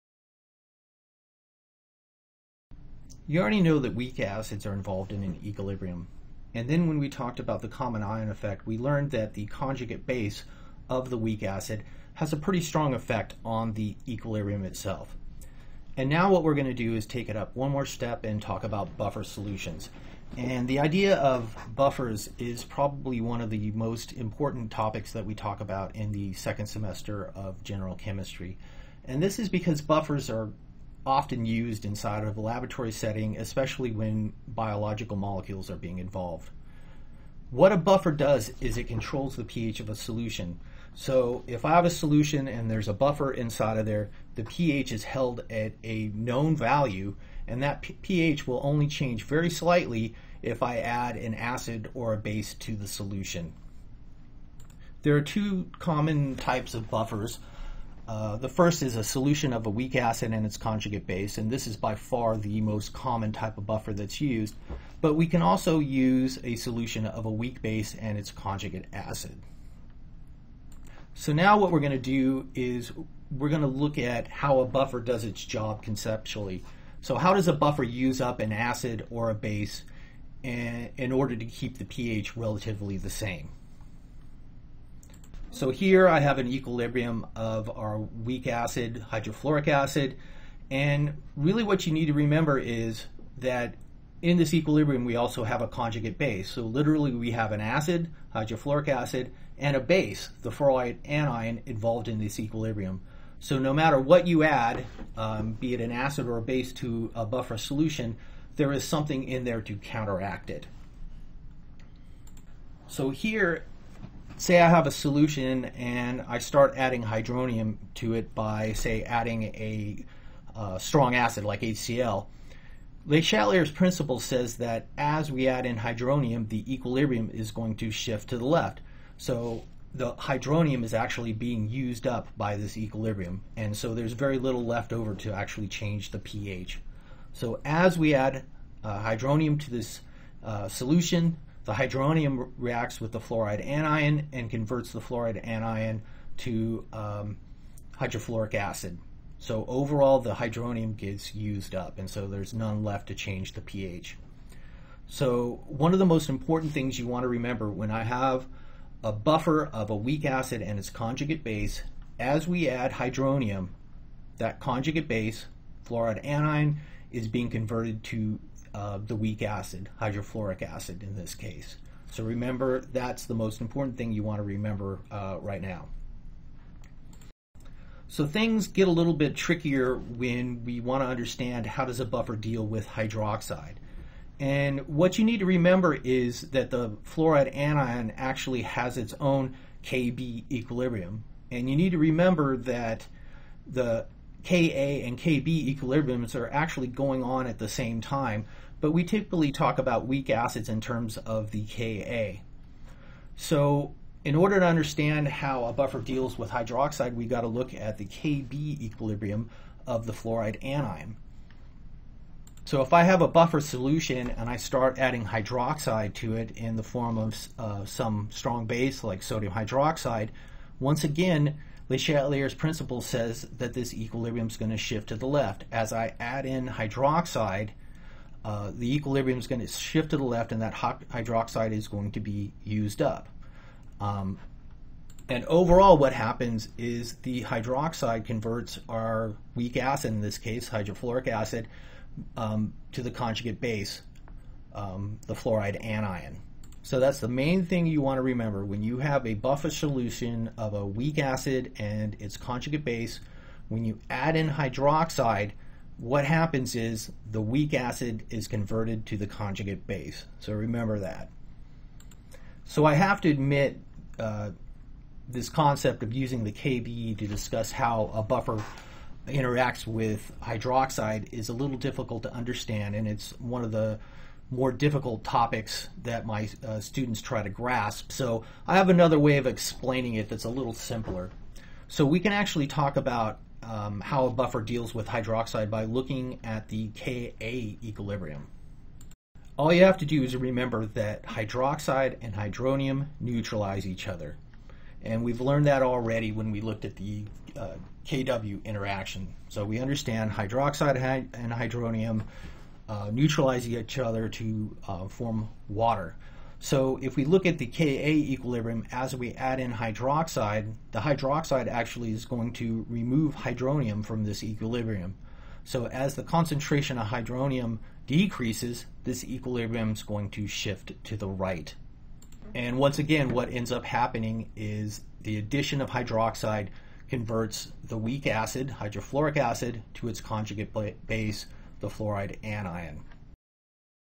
Determining the pH in Buffer Solutions
In a buffer solution, the pH values change very slightly with the addition of an acid or a base.
Buffers are used to control the pH of a solution.
This video discusses how Le Chatlier's principal allows a buffer to uses use up any H3O+ or –OH added to only provide a slight change to the pH.

Here is an index of the other videos in Chapter 19: Buffers & Titrations